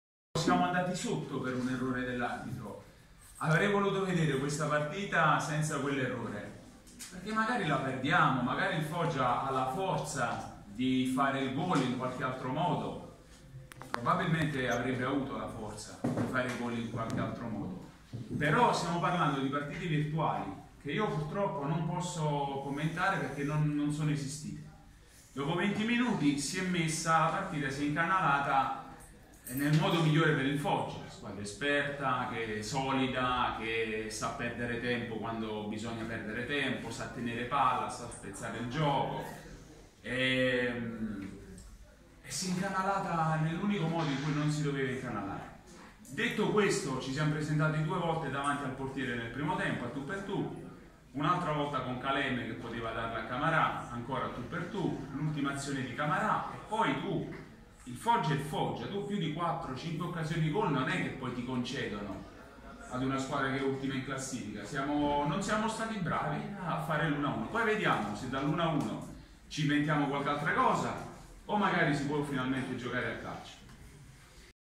Nardò - Foggia 0-1, l'allenatore dei salentini recrimina sull'episodio del gol dei rossoneri
Foglia Manzillo: ""Avrei voluto vedere un'altra partita senza l'errore dell'arbitro. Nonostante questo abbiamo avuto due grandi occasioni e magari sull'1-1 si sarebbe finalmente giocato a calcio"